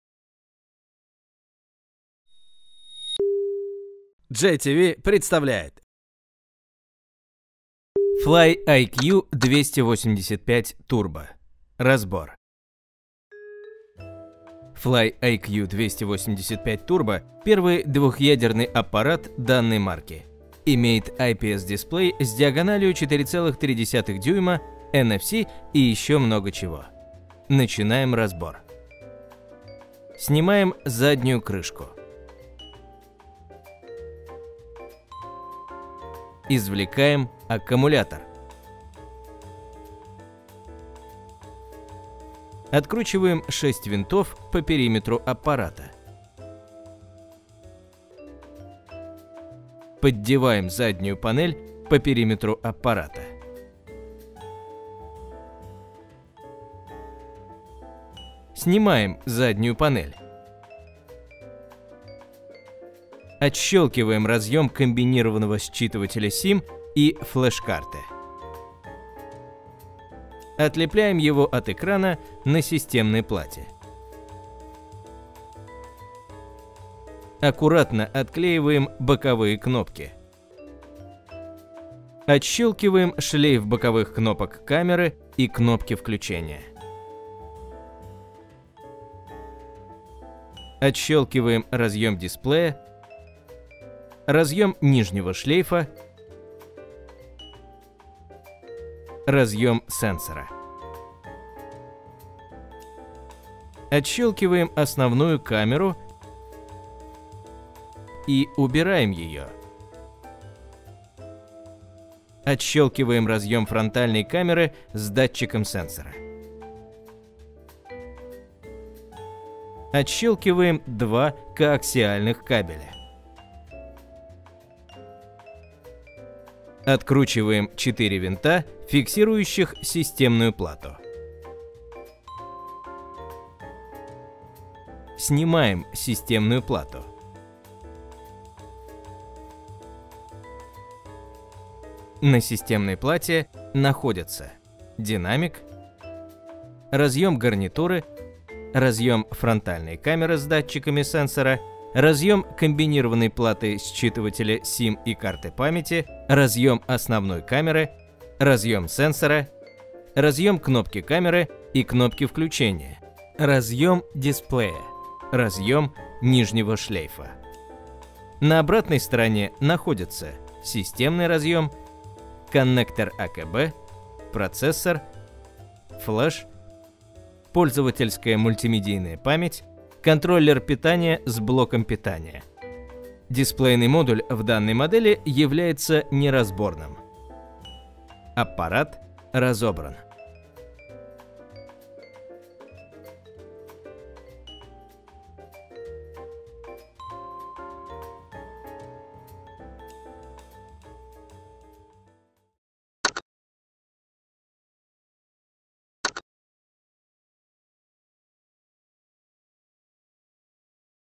Fly Turbo IQ285 - как разобрать смартфон и из чего он состоит
Ремонт телефонов Fly.

Технический обзор с описанием разбора мобильного телефона Fly Turbo IQ285: как разобрать Флай Турбо IQ285 своими руками и произвести ремонт. Канал GTV